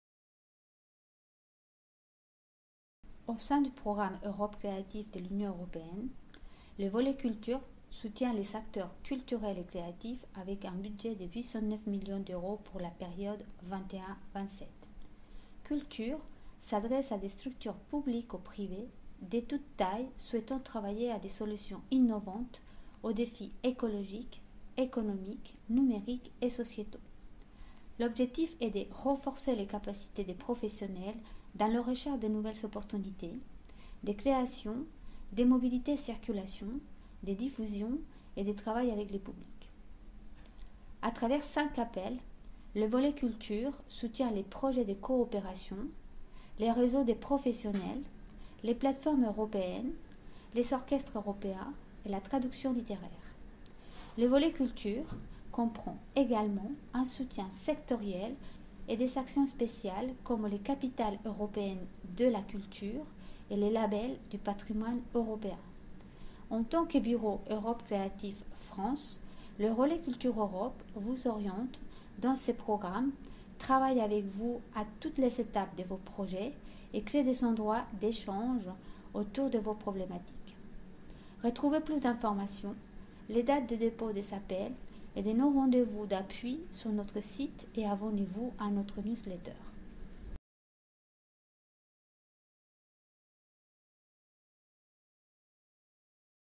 Europe Créative 2021-2027 : comprendre le volet Culture
À qui s'adresse le volet Culture du programme Europe Créative ? Quel est son budget pour la période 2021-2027 ? Quels secteurs sont soutenus à travers ce volet ? Comment fonctionnent les appels à projets ?
À travers cette vidéo, nous vous proposons de décrypter le volet Culture du programme Europe Créative en moins de 2 minutes !

📍 Suivez Relais Culture Europe !